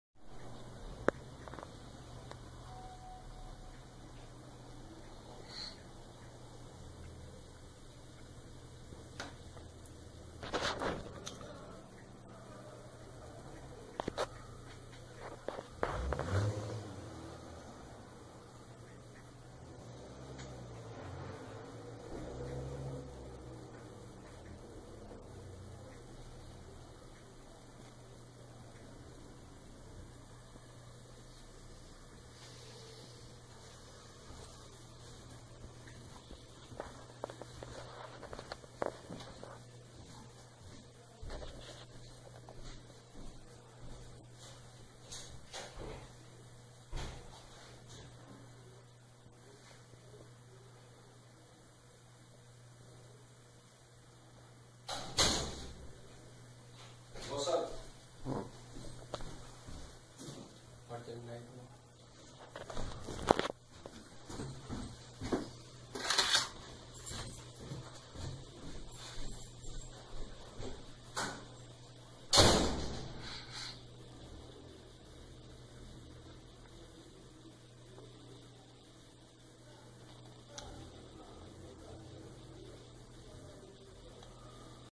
Ação dos ladrões arrombando o banco em Cerro Grande do Sul
Momento em os ladrões chegam a agência bancaria , entram e colocam os explosivos e após  saem com refém